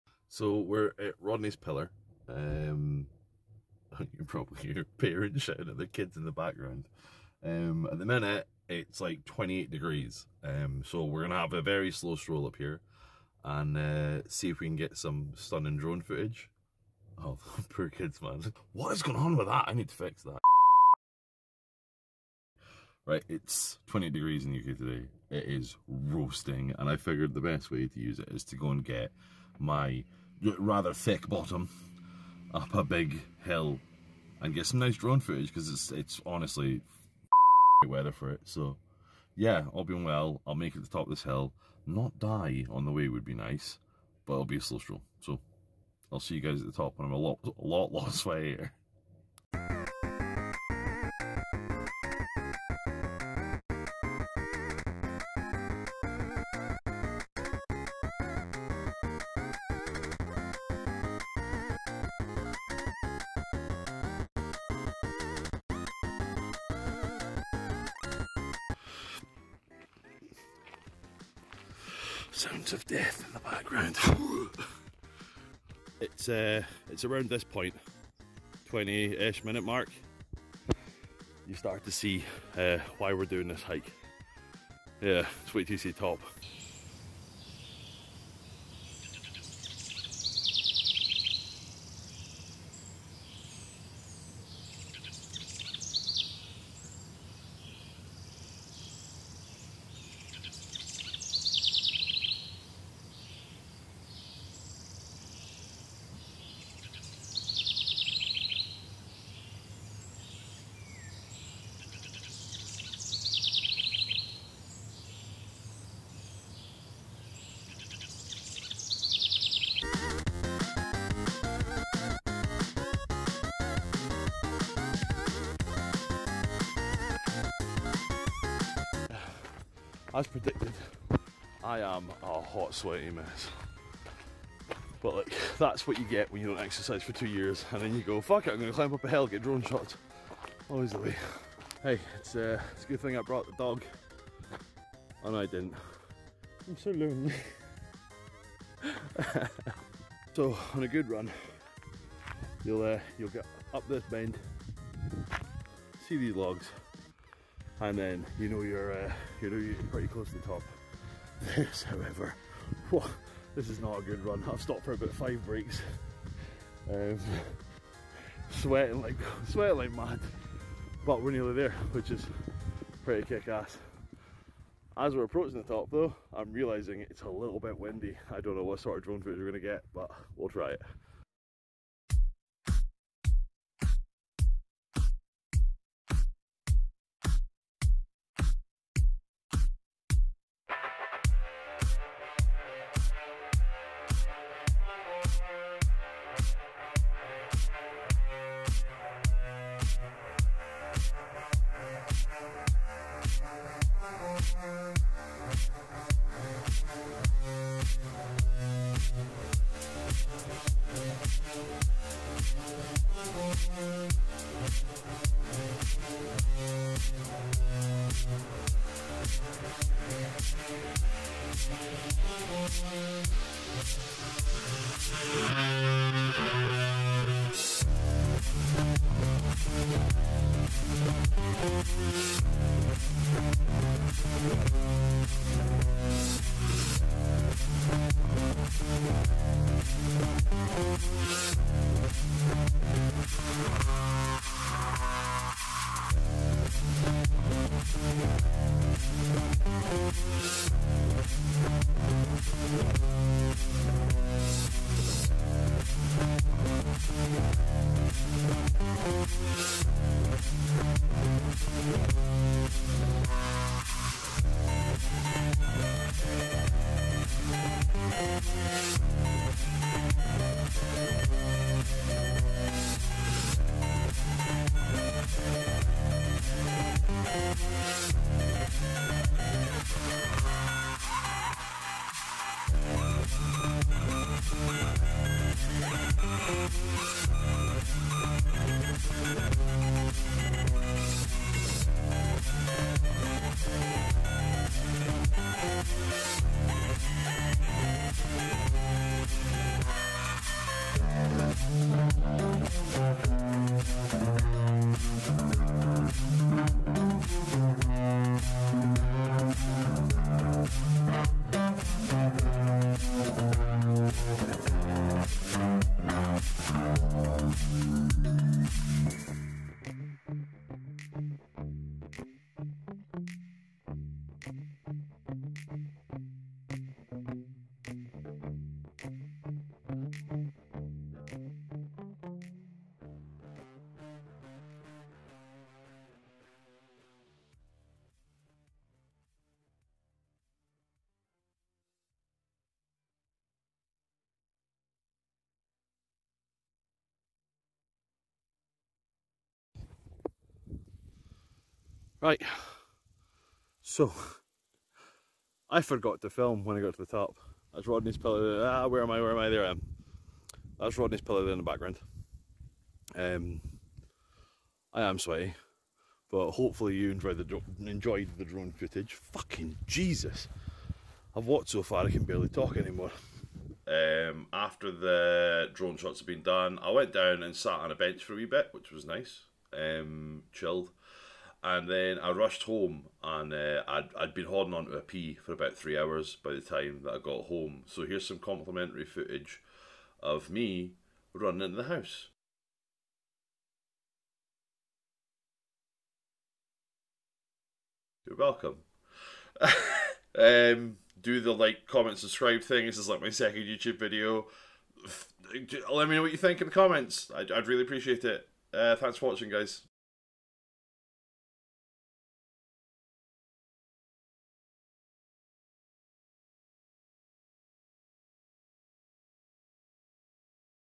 Adventure 1: Rodneys Pillar
I'm new to the whole drone thing and walking thing... and exercise thing...
I hope you enjoy this video as much as I enjoyed making it! (Also please ignore the swears I forgot to remove. Ooops!